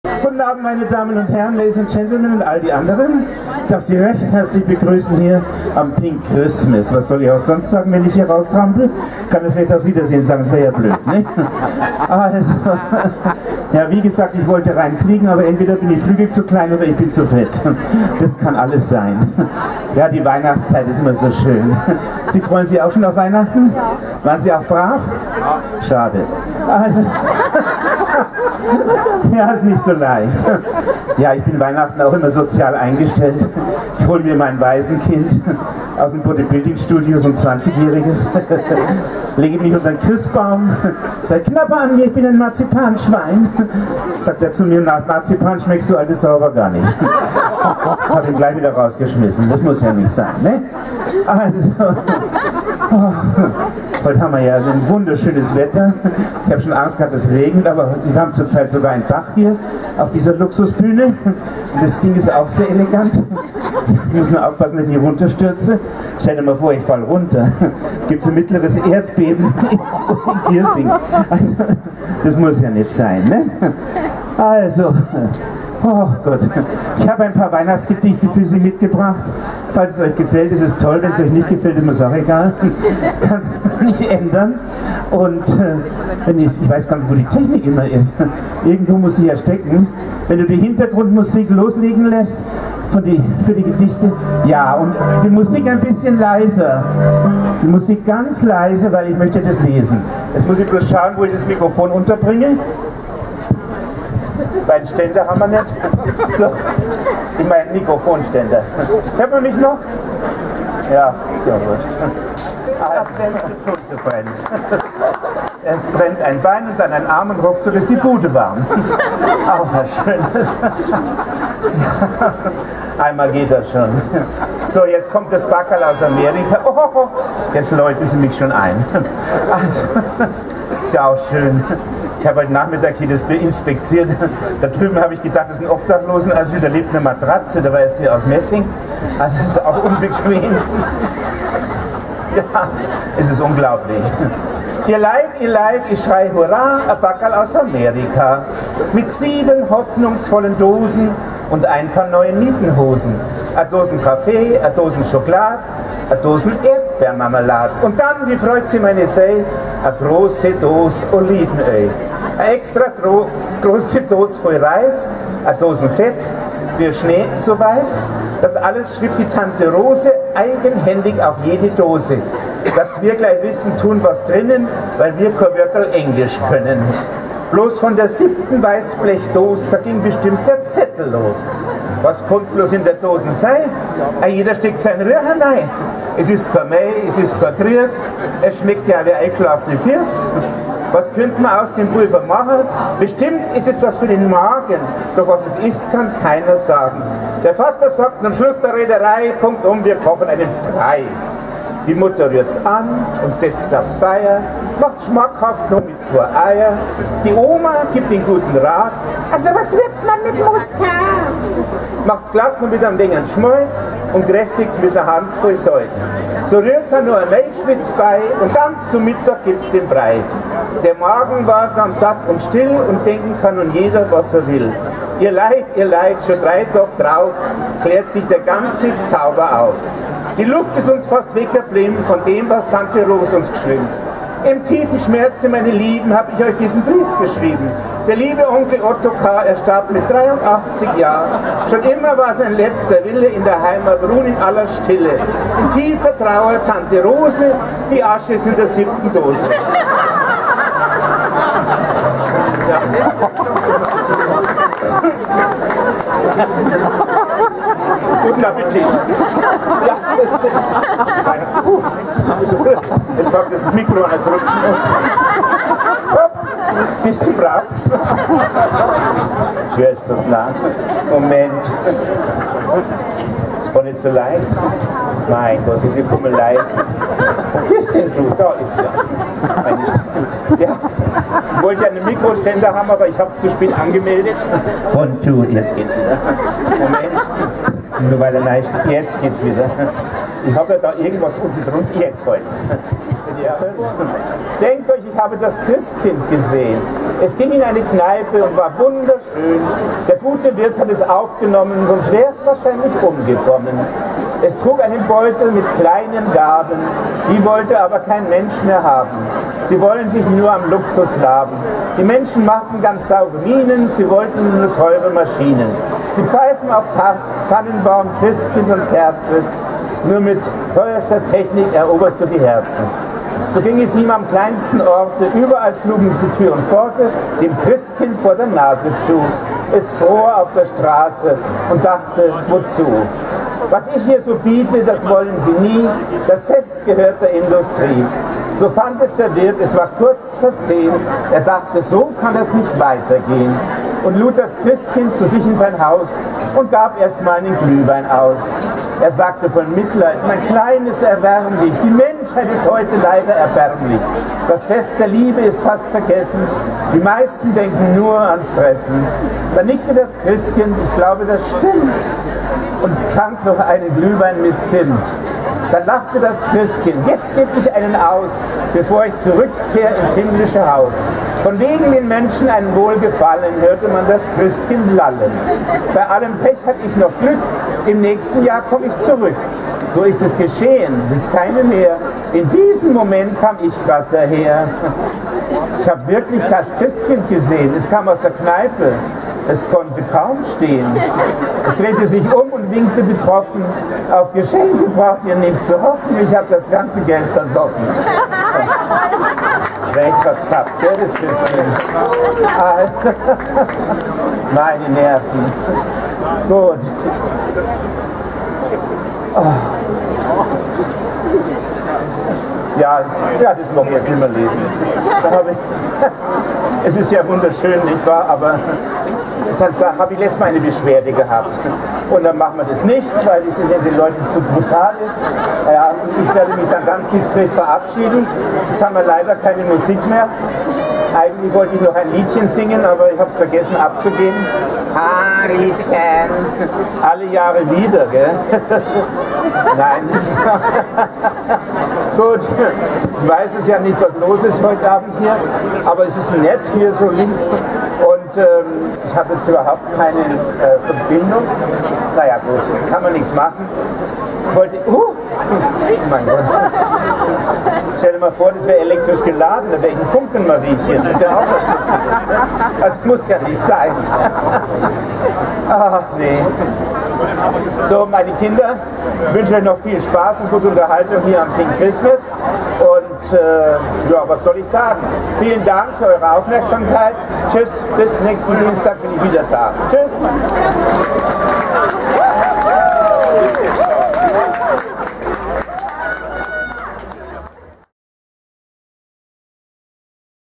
Petra Dorèn- Comedy, die "Queen Mum" of Munich auf dem "Pink Christmas"
Petra Dorèn ist ein Travestie- Star der ersten Stunde. Sie unterhielt ihr Publikum in der Schwulenszene in Zeiten, wo Homosexualität in Deutschland noch verfolgt wurde. Wer sich hierzu näher informieren möchte, kann hier weiterlesen:

Ich habe zwei lustige Geschichten gefilmt "Ein Päckchen aus Amerika" & "Denkt Euch, ich habe das Christkind geseh'n".
Viel Spaß!